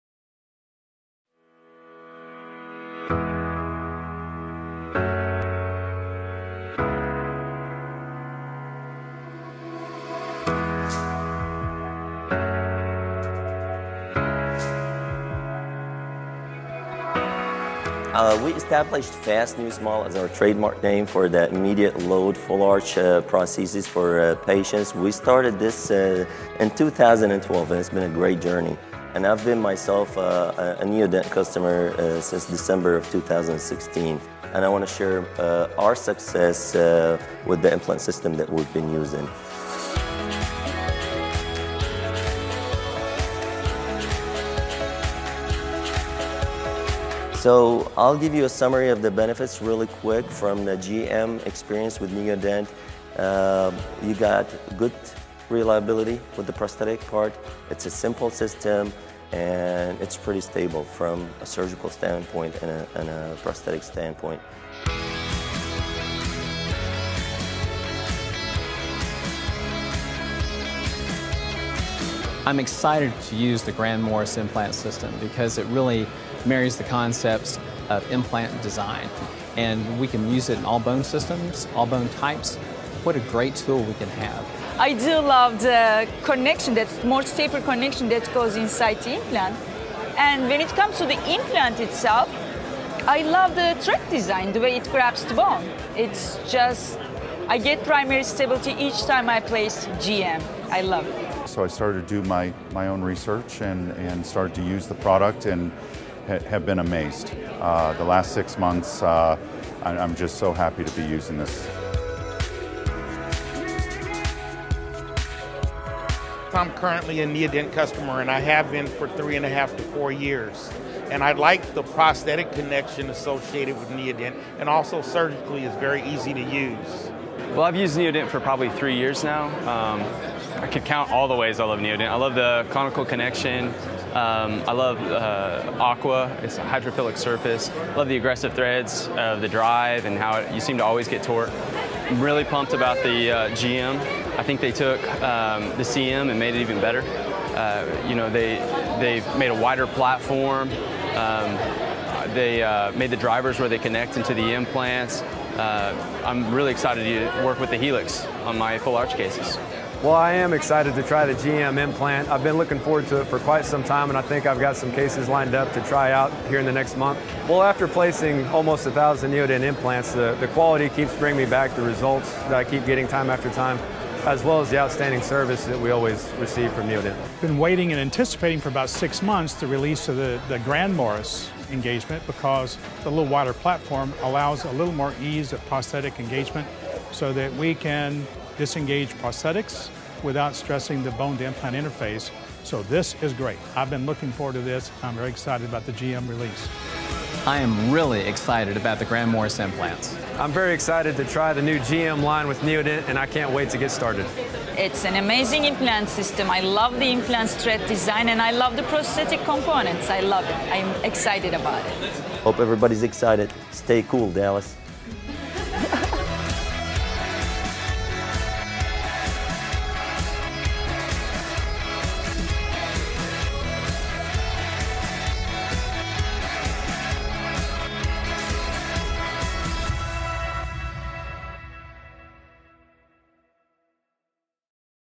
Neodent Grand Morse Launch
GRAND History was made in Dallas and now everyone can be a part of this new found success!  Take an aggressive, double threaded Implant from Neodent, bought by Straumann Group, add a morse taper connection with a platform switch, then add over 175 different restorative solutions and create the GM Implant System with the Helix implant.